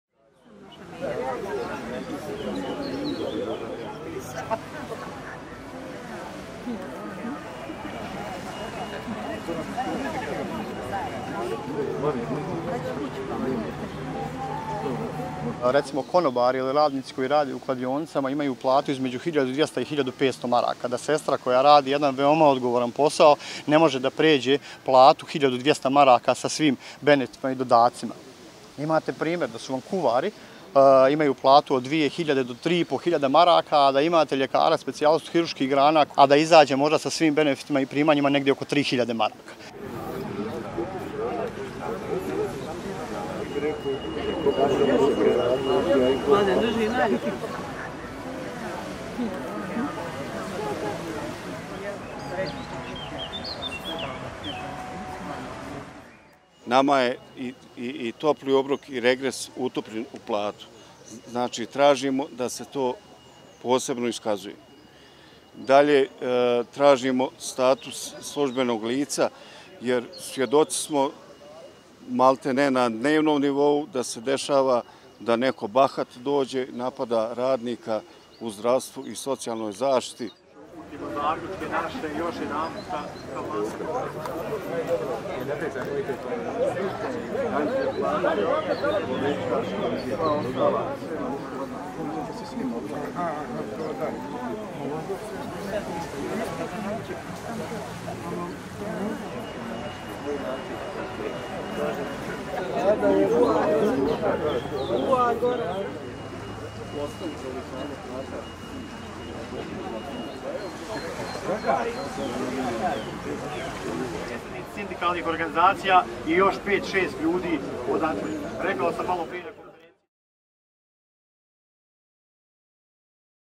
Veće plate i status službenog lica zatražili medicinski radnici sa protesta u Banjaluci
Zdravstveni radnici iz Republike Srpske na protestu upozorenja u Banjaluci zatražili su povećanje osnovne cijene rada sa 150 na 200 maraka (sa 75 na 100 evra). Traže i da naknade za topli obrok i regres ne budu dio osnovne plate, te da dobiju status službenog lica.